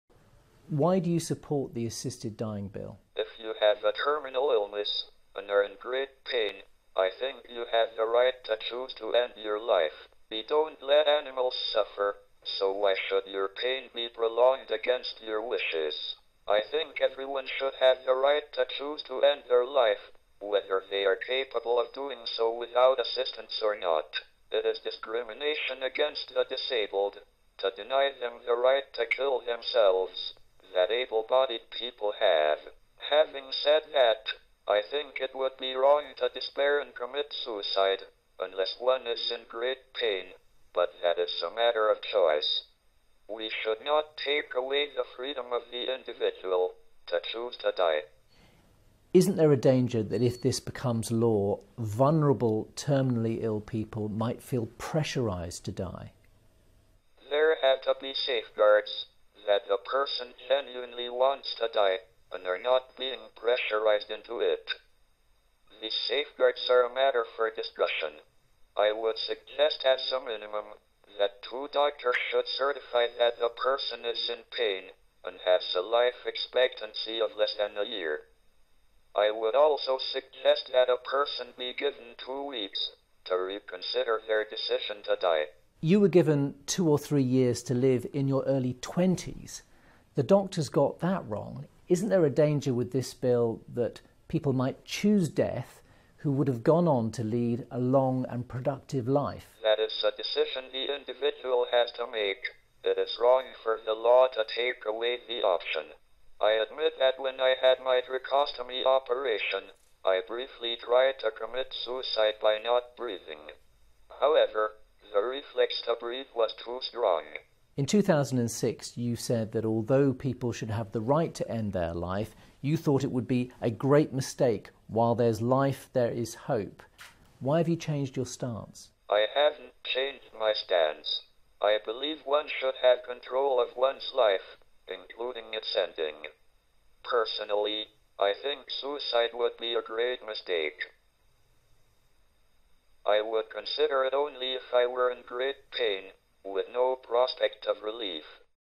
Hawking 'I support Assisted Dying'
Stephen William Hawking , né le 8 janvier 1942 à Oxford et mort le 14 mars 2018 à Cambridge, est un physicien théoricien et cosmologiste britannique. Théoricien de renommée mondiale, ses livres et ses apparitions publiques ont fait de lui une célébrité.

Depuis l'âge d'une vingtaine d'années, Hawking souffre d'une forme rare — de début précoce et d'évolution lente — de sclérose latérale amyotrophique (SLA) ; sa maladie progresse au fil des ans au point de le laisser presque complètement paralysé. Pourtant, il est professeur de mathématiques à l'université de Cambridge de 1980 à 20092, membre du Gonville and Caius College et chercheur distingué du Perimeter Institute for Theoretical Physics. Il est connu pour ses contributions dans les domaines de la cosmologie et la gravité quantique, en particulier dans le cadre des trous noirs. Son succès est également lié à ses ouvrages de vulgarisation scientifique dans lesquels il discute de ses théories et de la cosmologie en général, en particulier Une brève histoire du temps.

La clé des principaux travaux scientifiques de Stephen Hawking est fondée, en collaboration avec Roger Penrose, sur l'élaboration des théorèmes sur les singularités dans le cadre de la relativité générale, et la prédiction théorique que les trous noirs devraient émettre ce qui est aujourd'hui connu sous le nom de rayonnement de Hawking